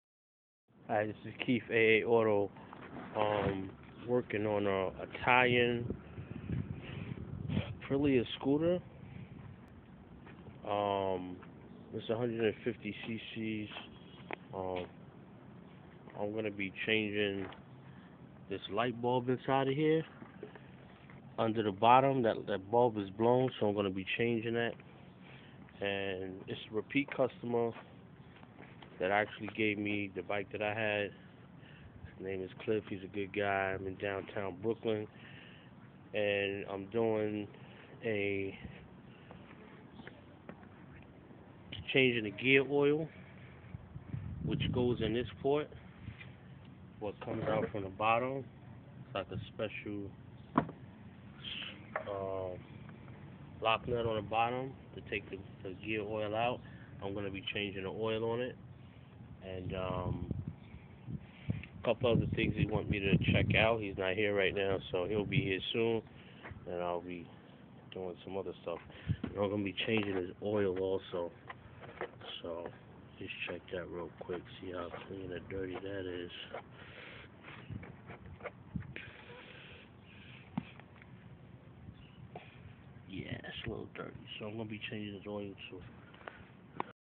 Changing gear oil on a 2008 apprillia scooter
changing motor oil gear oil head light bulb. keith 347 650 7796